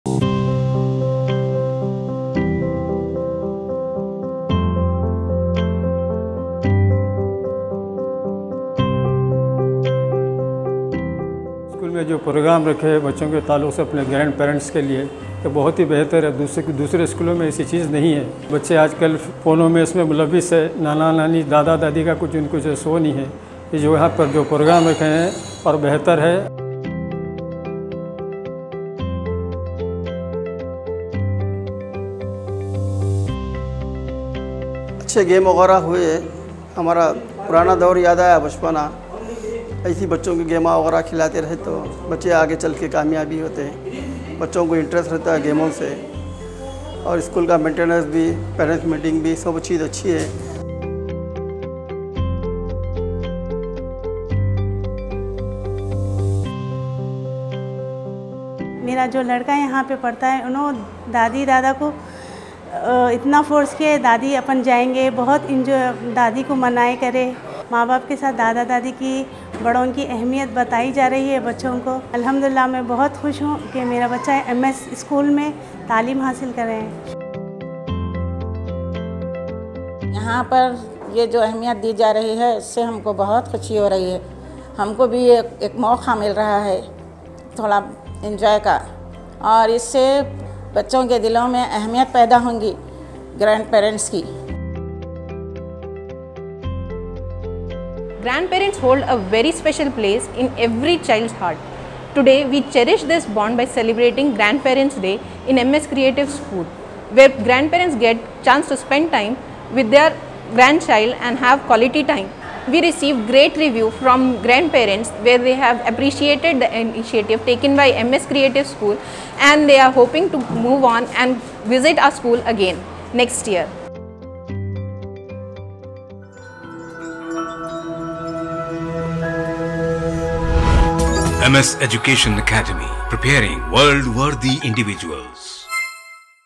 Grandparents Day Celebration at MS Creative Kids | A Heartwarming Tribute Across Generations
Join us as we celebrate the special bond between grandparents and their grandkids at MS Creative Kids! This heartwarming event is filled with laughter, love, and fun activities that bridge the gap between generations. Watch as paper cups take flight, pompoms find their place, and cherished moments are created. Don't miss this touching tribute to the timeless connection between elders and the young, where wisdom, love, and joy come together in perfect harmony.

© MS Education Academy-2023
Any reproduction of this content will result in legal action